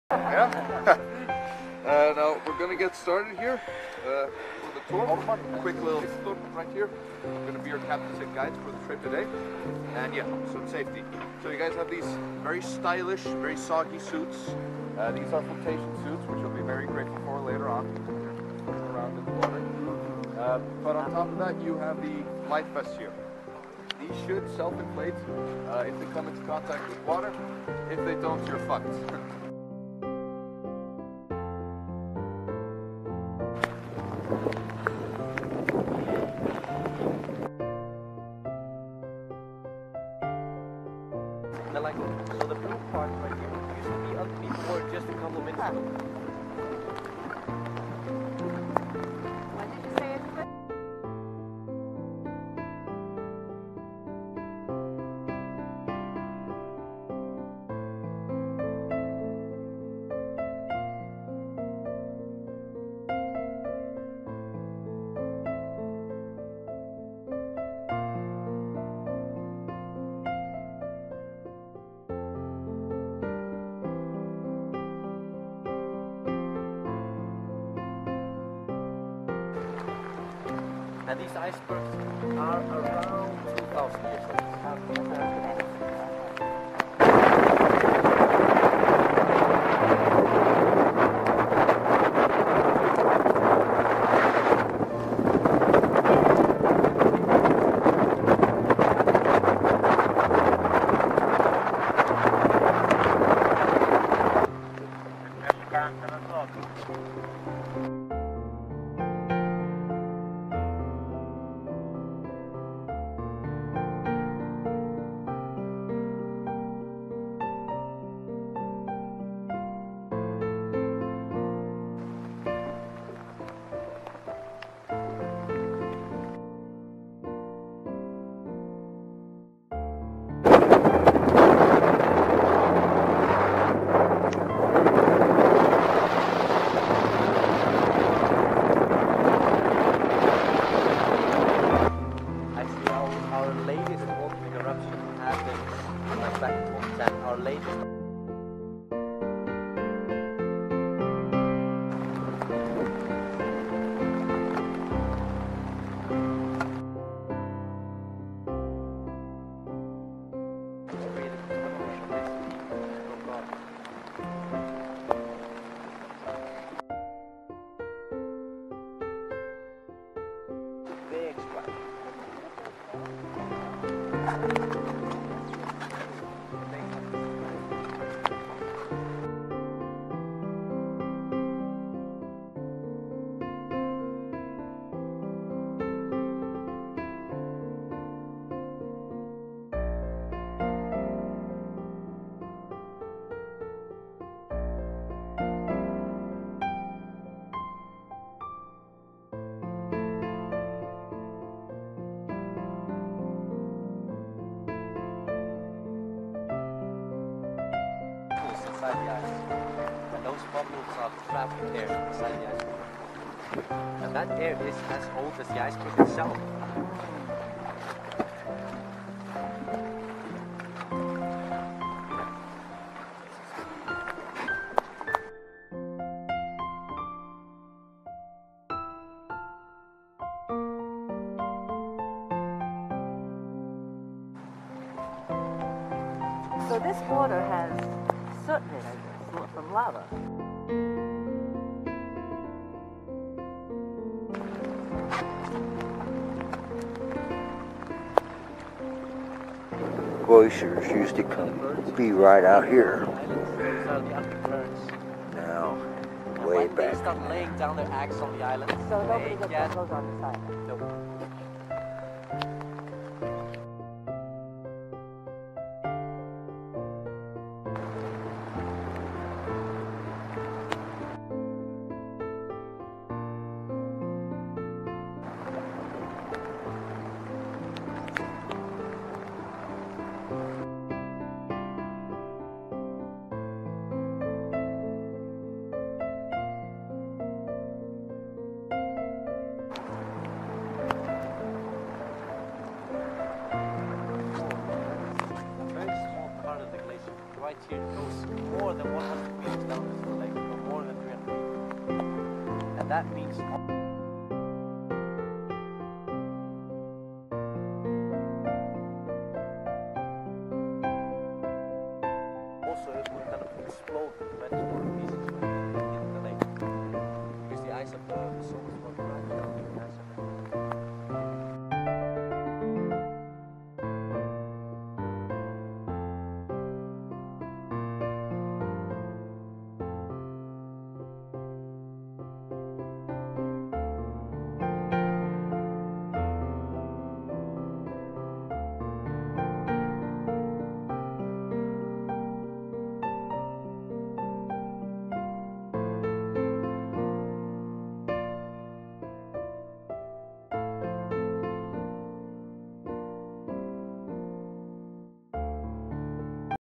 Iceland 2024 June Zodiac glacier iceberg tour
Prior to boarding our NCL cruise we rented a car and drove the 1S to view the South side of Iceland seems that the Vikings never knew of the Celtics on the South Iceland side understandable once you view the terrain.  Would NEVER had tried this while stationed here in 1966.  FEW english speaking back then now everybody.  Hope you Likey  camera assist for those too many steps by my youthful wife Millie.